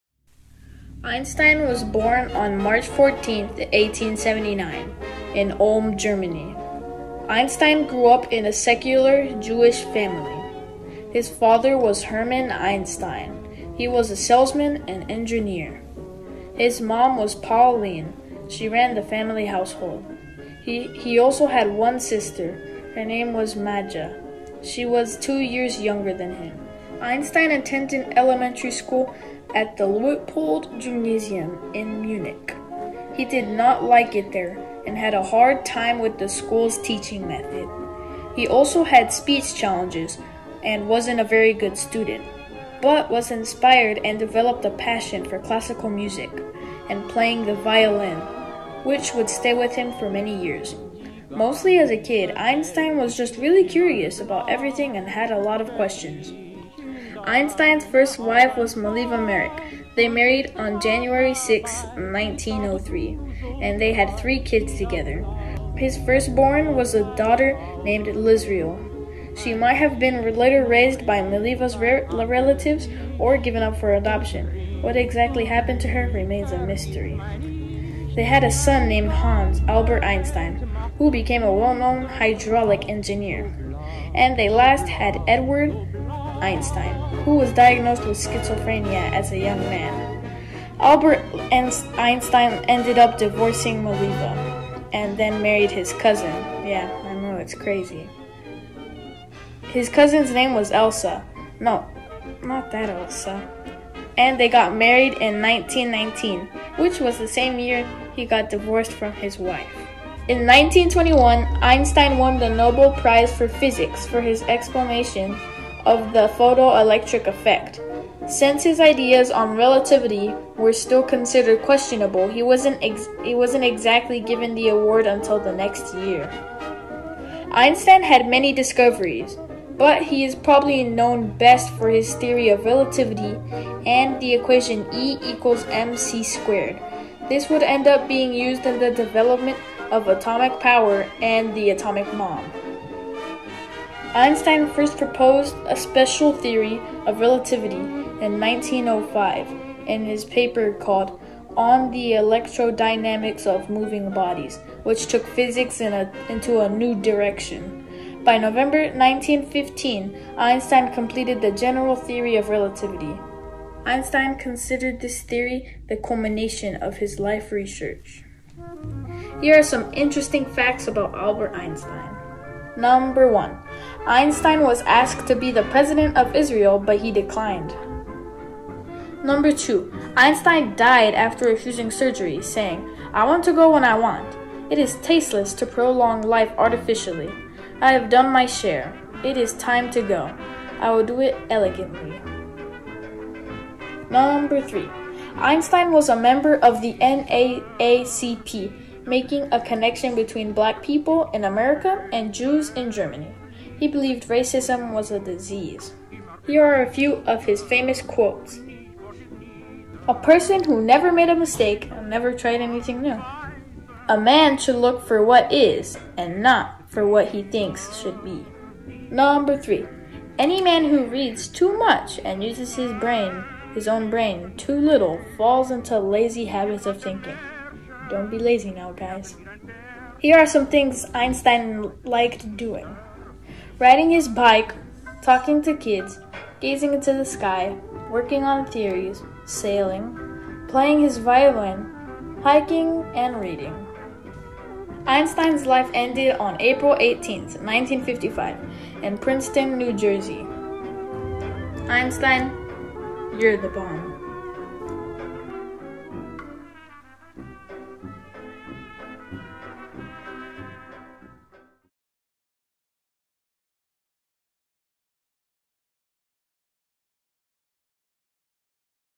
Albert Einstein By Manny Flores
Documentary of the life and death of Albert Einstein.